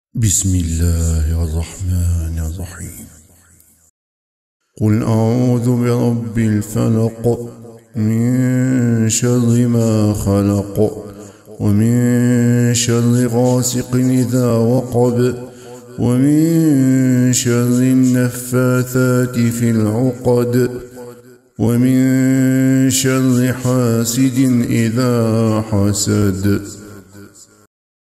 محمد حسن | سورة الفلق برواية ورش عن نافع | جودة عالية HQ
ثلاوة مباركة لسورة الفلق للقارئ محمد حسن برواية ورش عن نافع